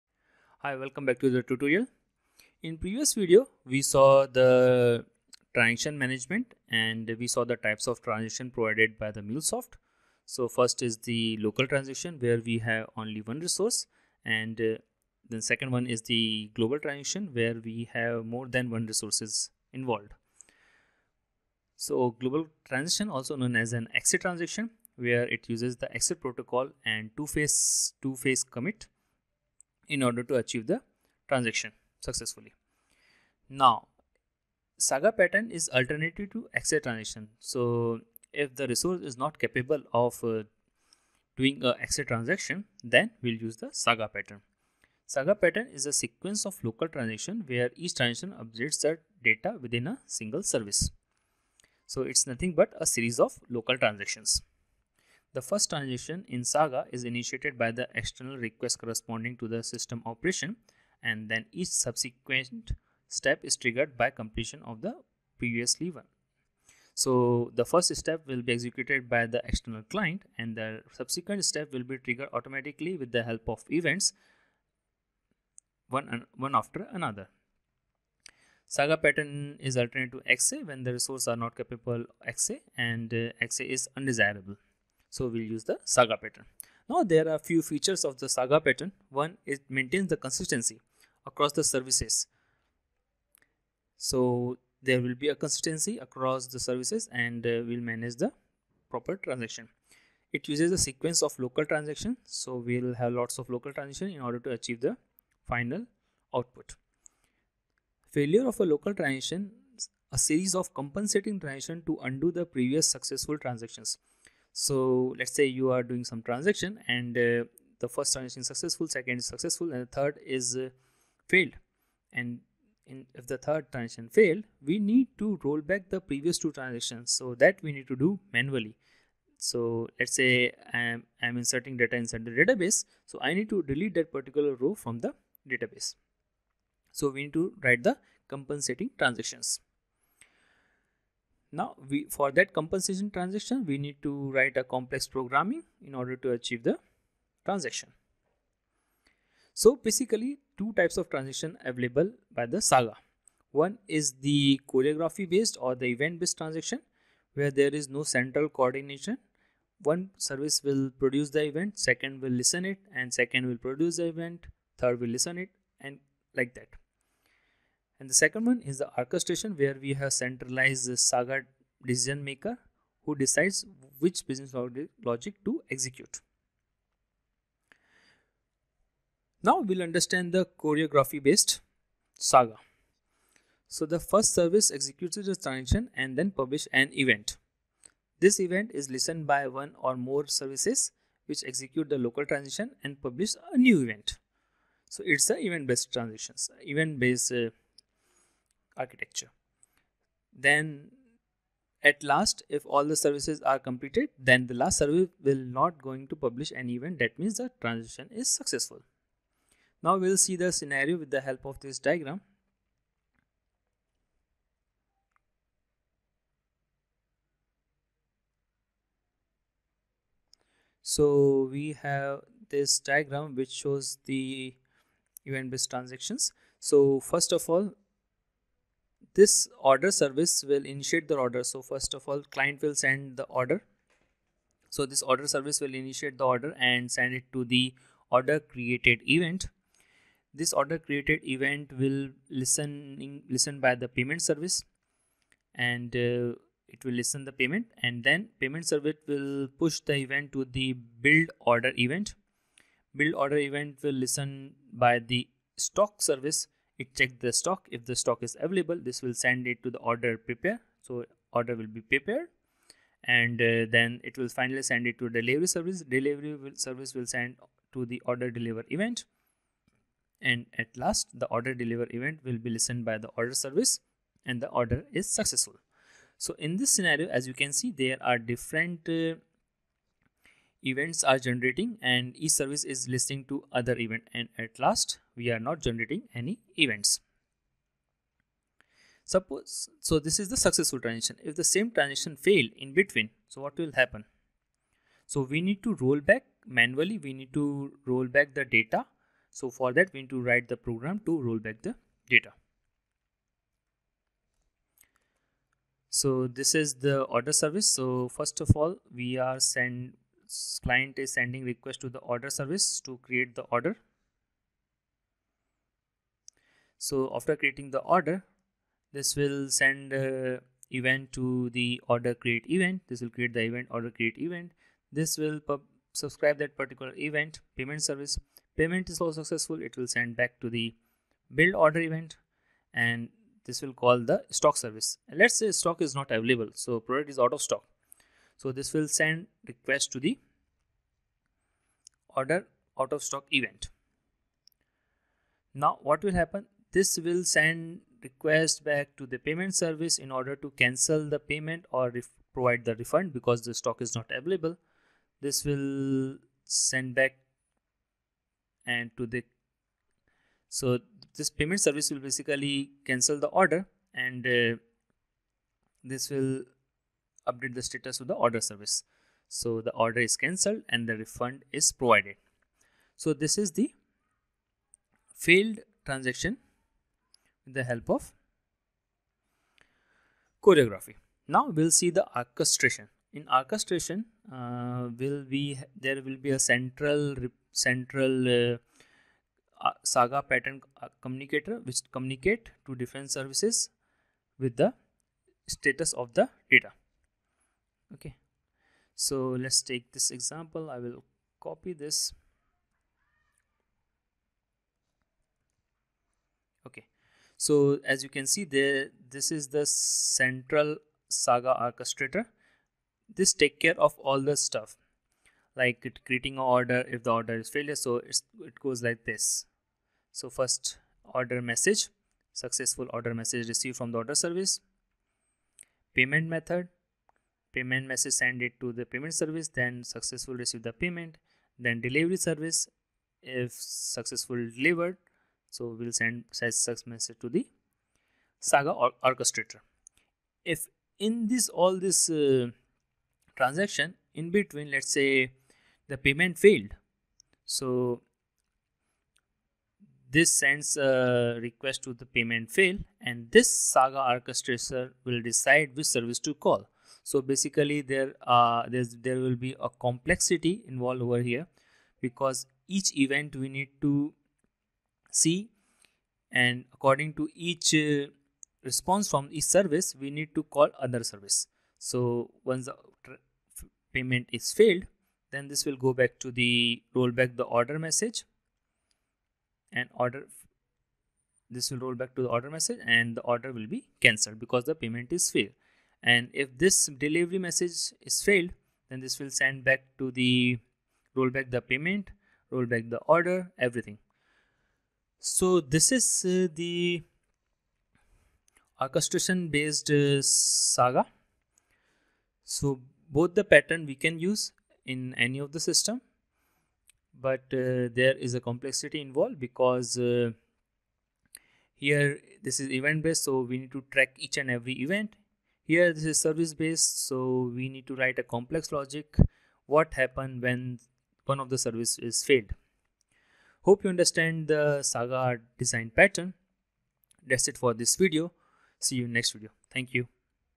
SAGA Pattern in Mulesoft (Choreography and Orchestration)
Saga Pattern in Mulesoft

Two types of Saga Pattern:
1) Choreography.